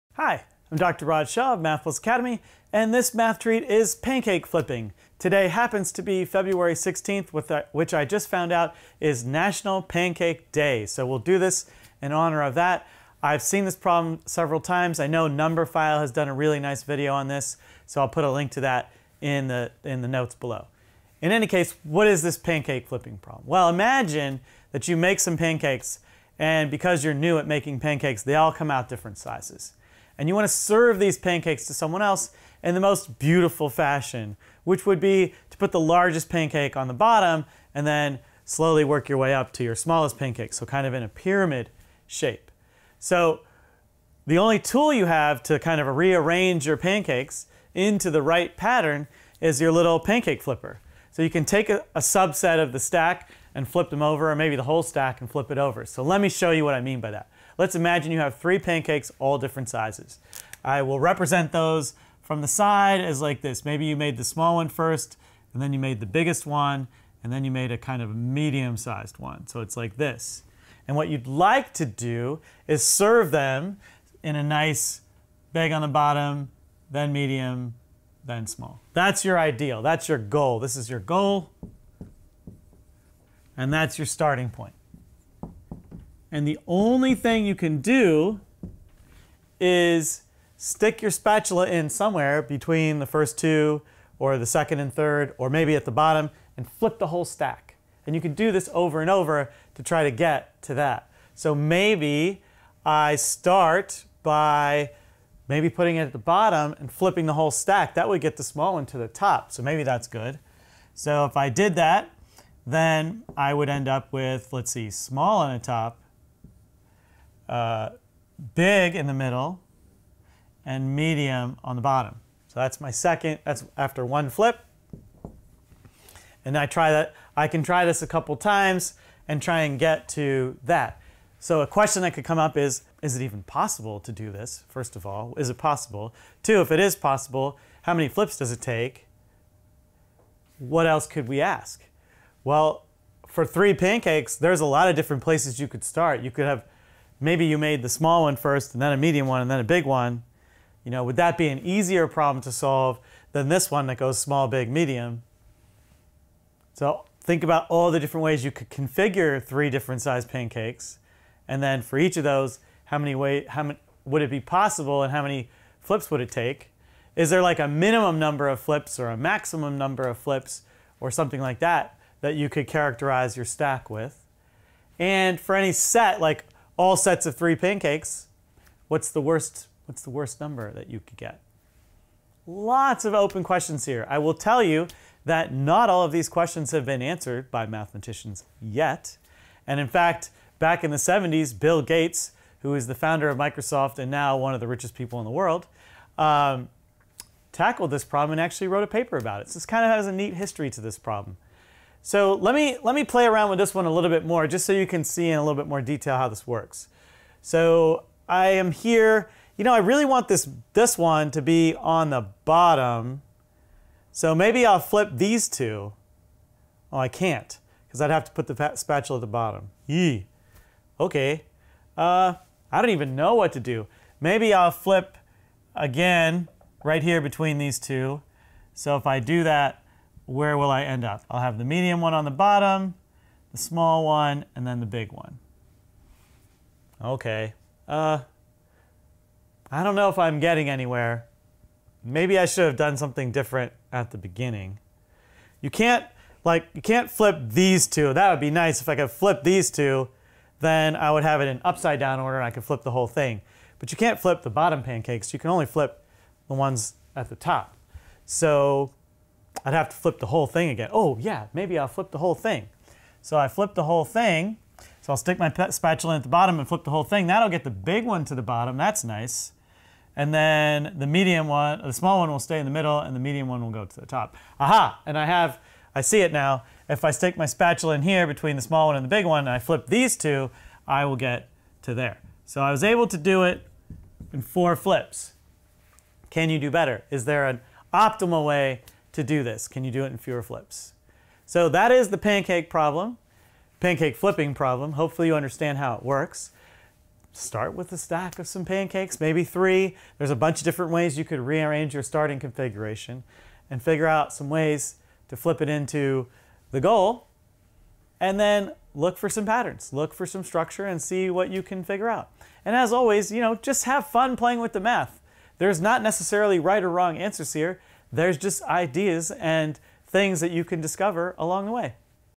Pancake Flipping
Imagine a stack of all different size (diameter) pancakes. Can you get them into a stack from largest to smallest? Rule: You can insert your flipper between any two pancakes (or at the bottom) and flip all the pancakes above the flipper. Is it always possible to flip them into a nice stack? Is there an optimal number of flips for every configuration of pancakes? Can you find any patterns or strategies to flip pancakes efficiently?